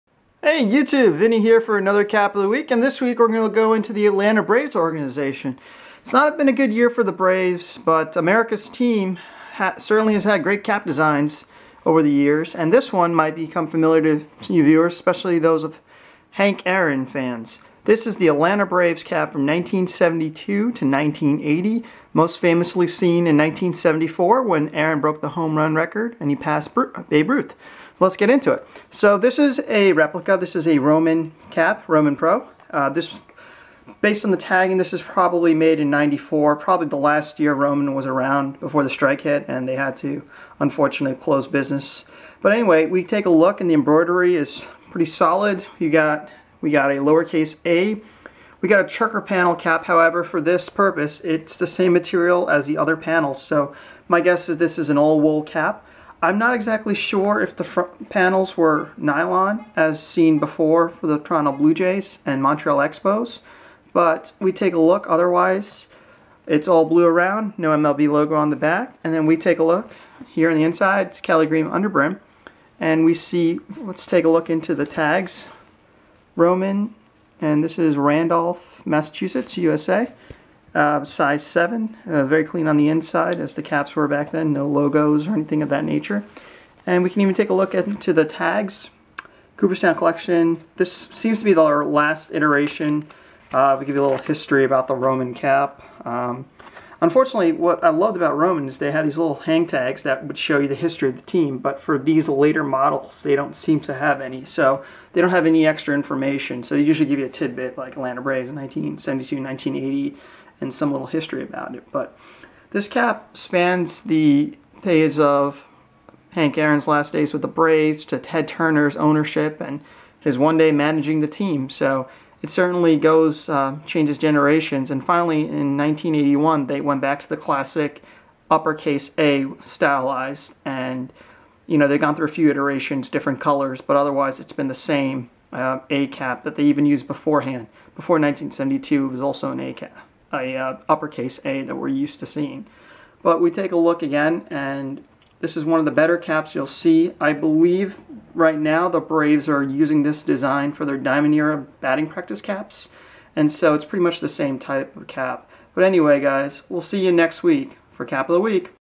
Cap of the Week: Atlanta Braves 1972-1980
Hi again YouTube, today's Cap of the Week is the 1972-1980 Atlanta Braves cap!

Made by Roman Pro as part of their Cooperstown Collection, this cap was most famously worn by Hank Aaron on April 8, 1974 when he hit his 715th Home Run, surpassing Babe Ruth to become the career leader!

The trucker-style cap features a lower-case "a" on the front and is currently being used as a batting practice cap by the Braves.

Come back each week for another Cap of the Week!